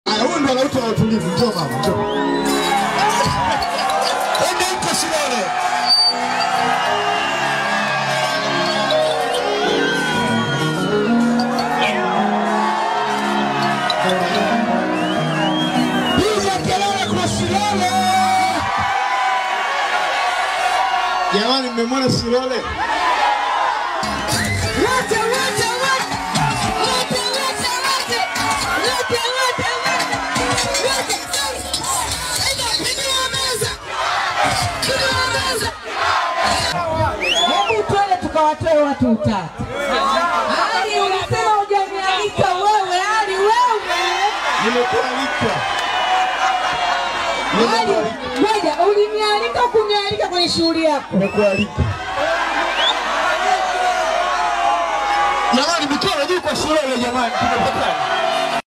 ALIKIBA , SHILOLE SASA MAMBO SAFI | WAMALIZA TOFAUTI ZAO KIGOMA
Alikiba usiku wa Jana alimpandisha Shilole stejini Na kumaliza Bifu lao.